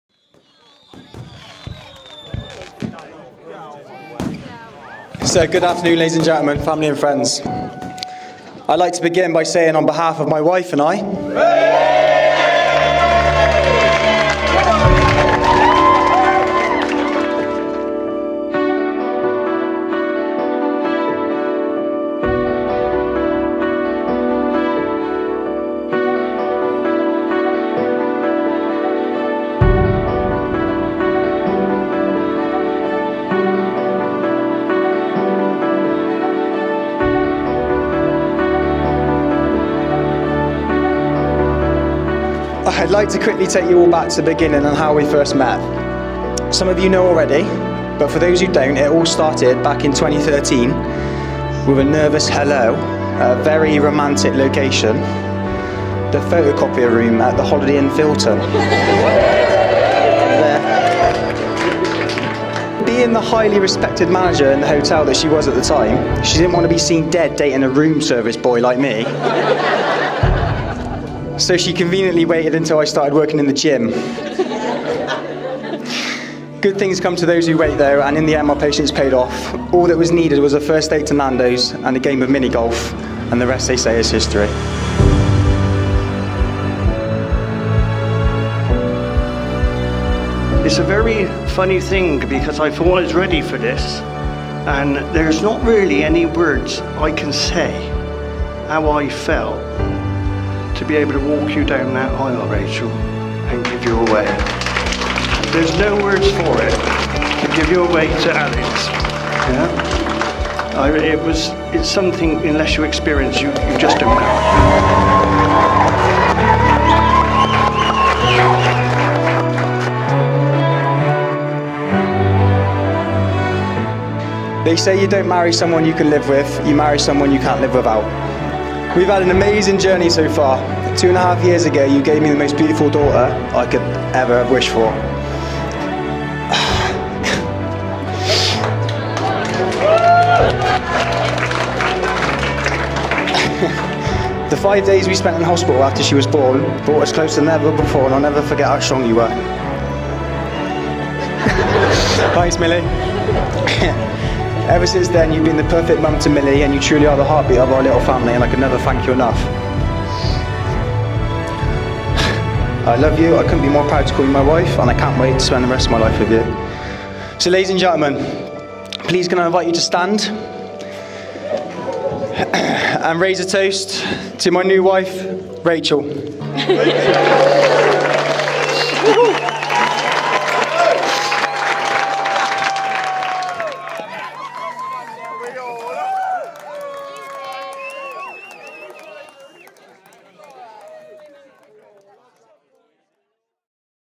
Quantock Lakes September Wedding
Rachelle & Alex September 2021 highlights video by Pristine Entertainment